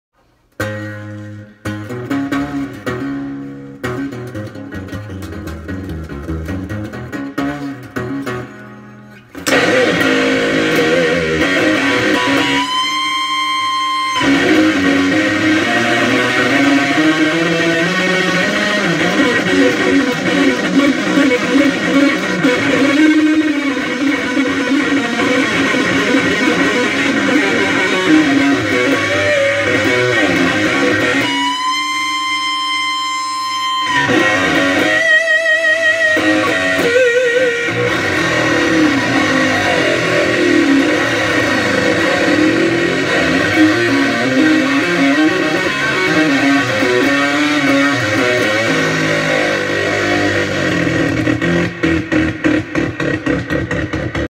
" Barney plays the blues " Kinley Wolfe
Playing the " blues " using a Kelley modded Metal Zone , I think it's as good as the Pussymelter .